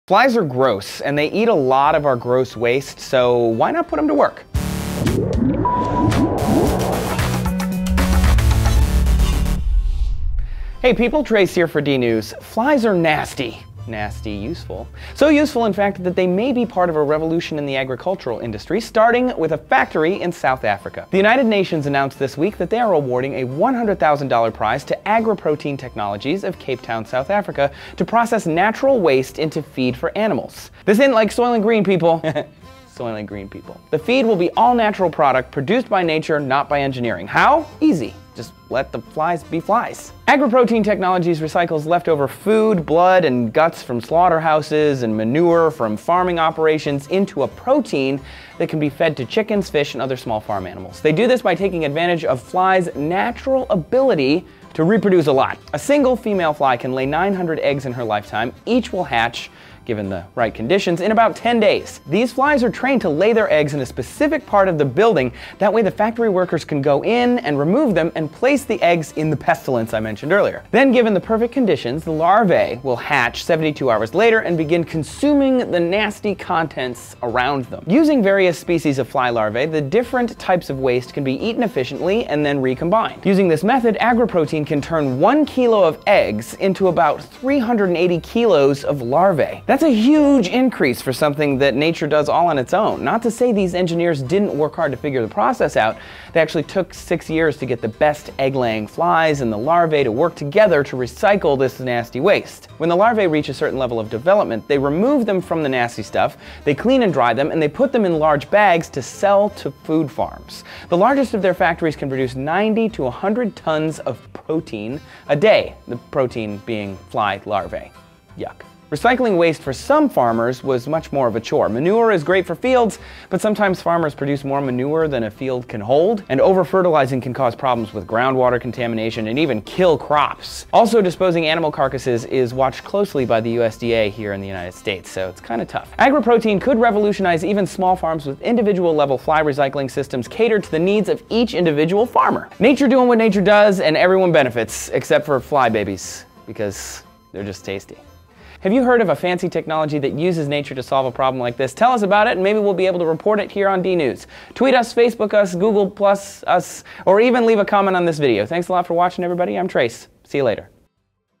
Fly Factory Turns Waste Into Food
That steak is some good eating! But for all the animal parts we do eat, there is a whole lotta stuff we don't. Trace recounts the amazing way flies are changing the way we clean up bloody, disgusting messes.

"A fly factory that transforms blood, guts, manure and discarded food into animal feed has walked away with a $100,000 UN-backed innovation prize."

"While there are certainly other flies and insects that make themselves at home in your home, 90 percent of all flies found inside are members of the specific species of fly musca domestica."

"Manure is a valuable source of organic matter and nutrients for gardens and crops. Manure should be applied to crops, pastures, and gardens based on plant nutrient needs. Don't guess, soil test."